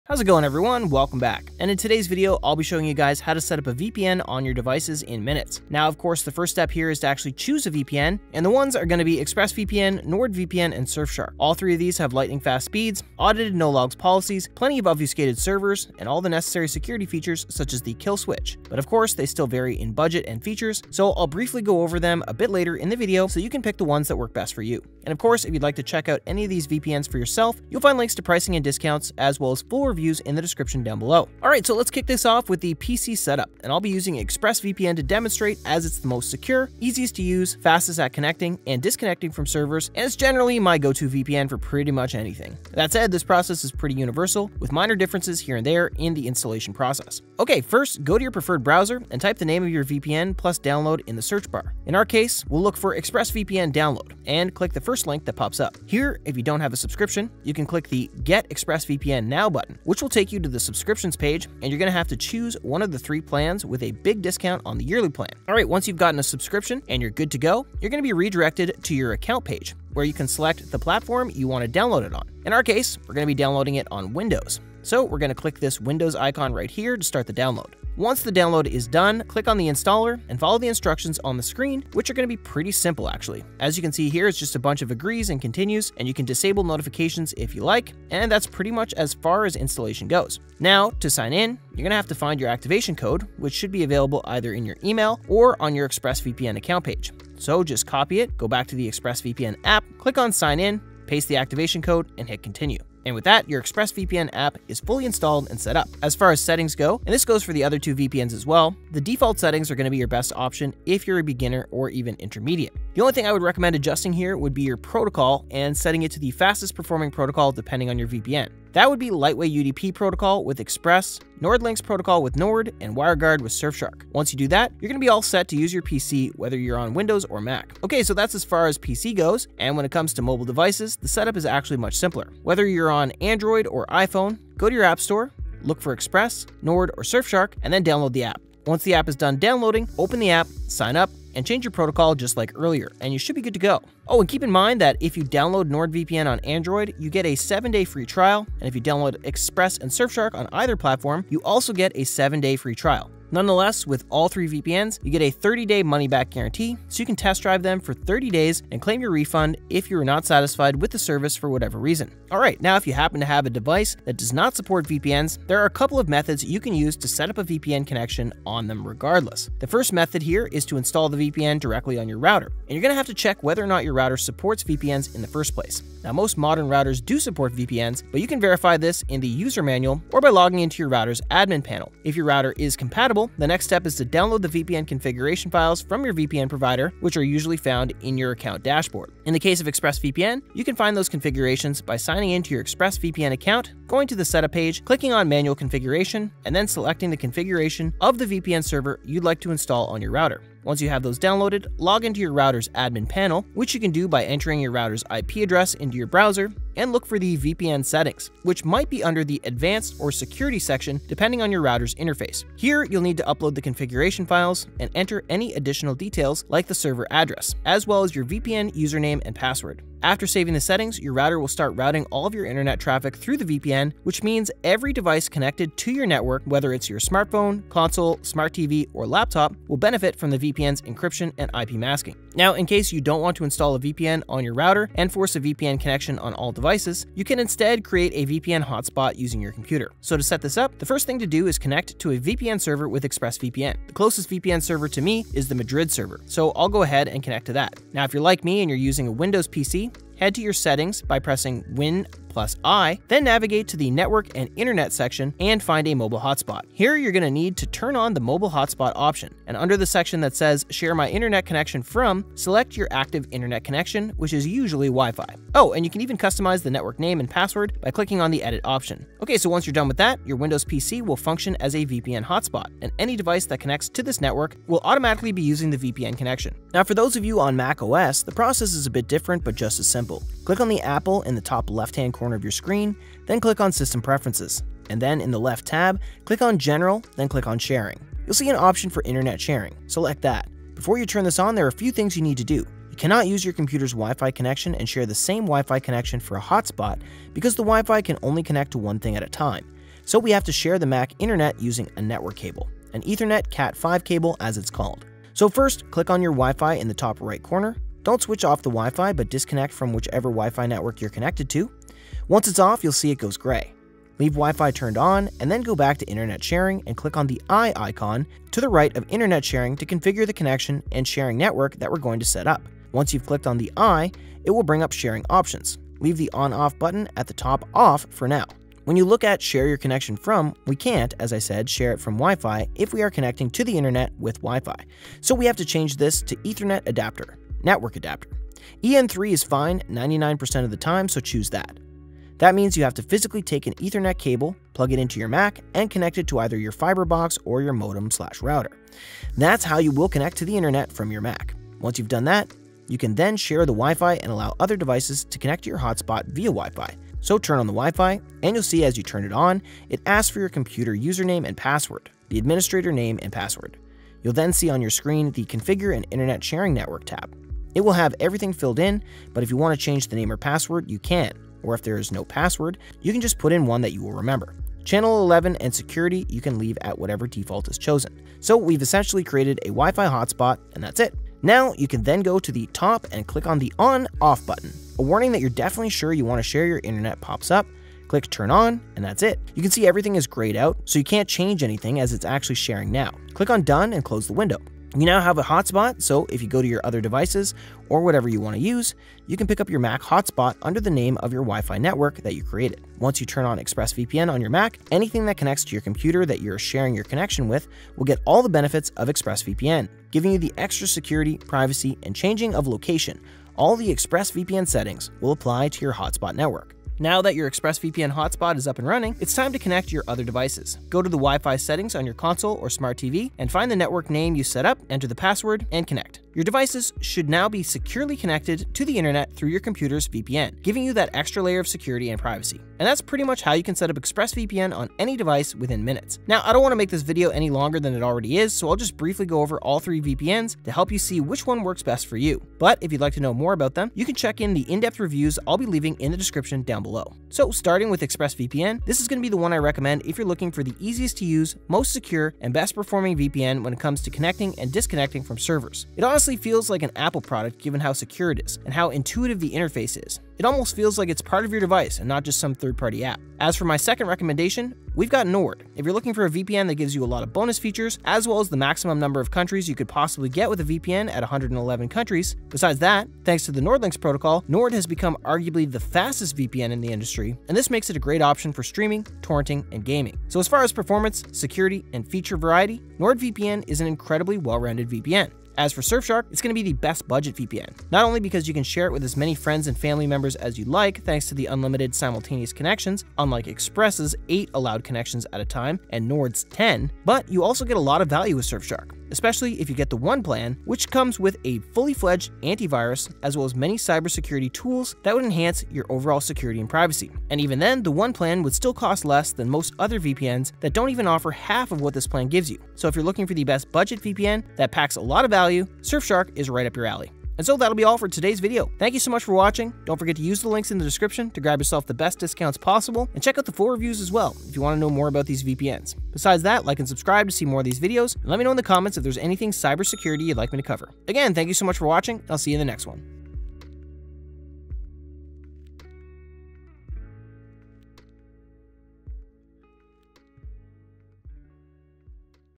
How to Set Up VPN within Minute: Flash Tutorial
🌐How to Setup VPN🌐

How to Set Up a VPN on Your Devices

In today's video, I’ll guide you through the quick and easy process of setting up a VPN on various devices. We’ll be focusing on three top options: ExpressVPN, NordVPN, and Surfshark. Each of these VPNs boasts lightning-fast speeds, audited no-logs policies, and essential security features like a kill switch. While they all excel in performance, they differ in pricing and specific features, which I’ll touch on later to help you make the best choice for your needs.

Setting Up VPN on PC and Mobile

Starting with PC, I’ll demonstrate the setup process using ExpressVPN, known for its user-friendliness and security. Once installed, activate it using your activation code and adjust the settings to use the optimal protocol for speed. For mobile devices, the process is even simpler—just download the VPN app from your app store, sign up, and customize your protocol. Plus, all three VPNs offer free trials and money-back guarantees, so you can test them out without commitment.

Advanced Setup for Non-Compatible Devices

For devices that don’t support VPNs directly, you can either set up the VPN on your router or create a VPN hotspot from your computer. If your router supports VPNs, download the configuration files from your VPN provider and upload them to your router’s admin panel. Alternatively, connect to a VPN server on your PC and share that connection as a hotspot. This way, any device that connects to your hotspot will benefit from the VPN’s encryption and privacy features, ensuring a secure browsing experience across your entire network.

Hope you enjoyed my How to Set Up VPN within Minute: Flash Tutorial Video.